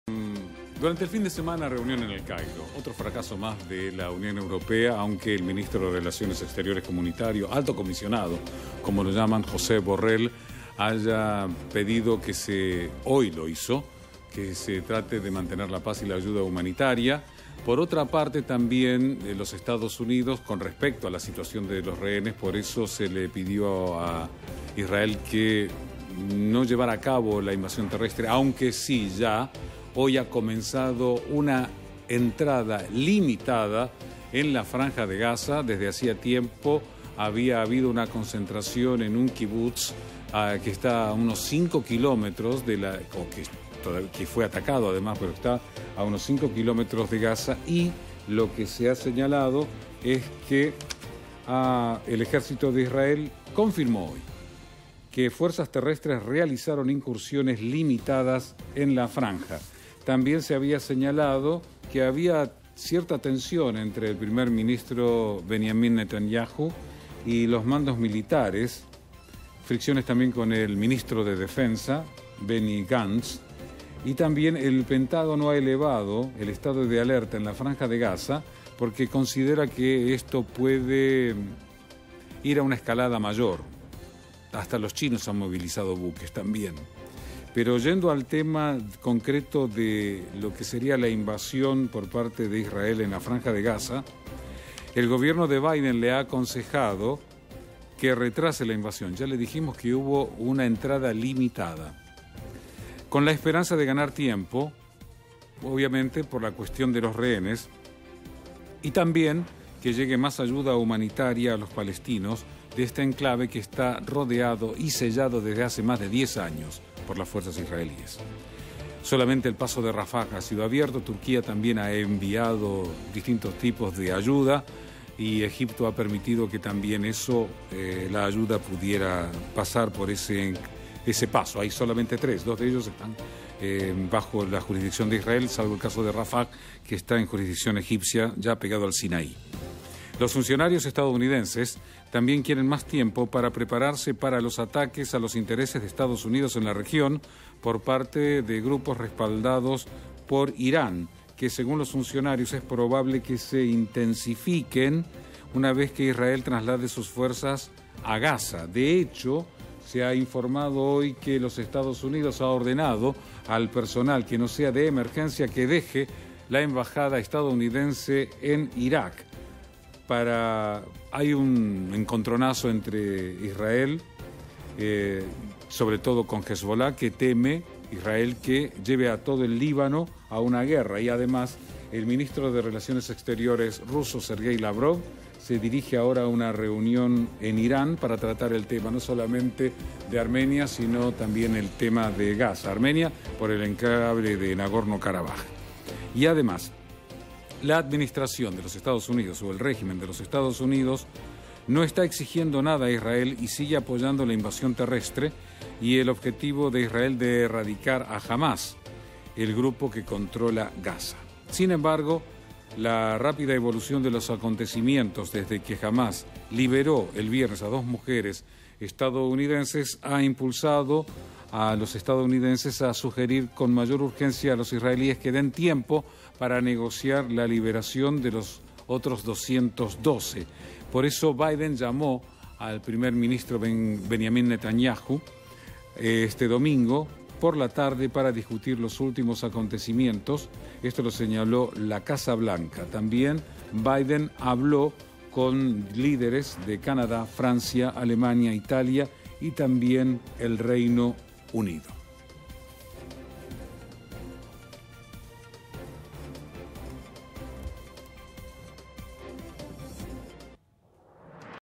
EEUU intenta ralentizar la invasión a Gaza
👉 El país norteamericano pretende que el avance de los israelíes sobre el territorio en disputa en Medio Oriente tenga un ritmo más lento para negociar por los rehenes capturados por Hamás.